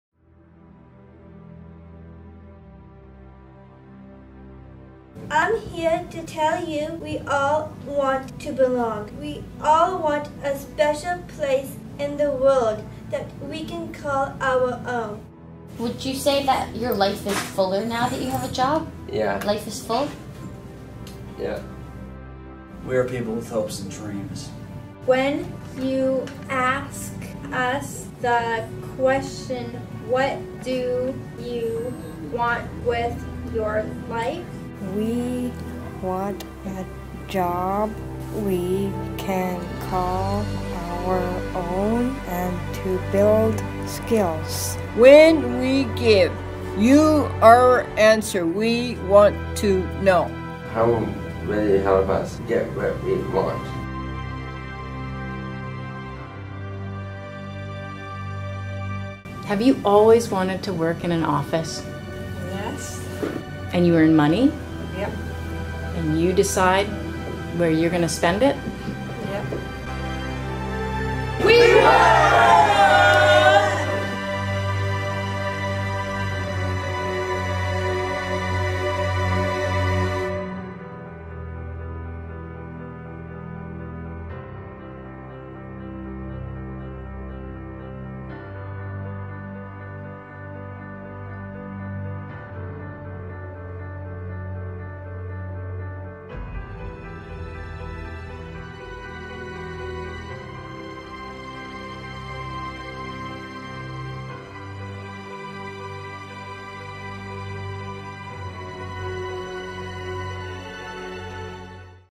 We Work!
By: Seven Southridge Students from White Rock, B.C. with Semiahmoo House Society Cusomized Employment Services.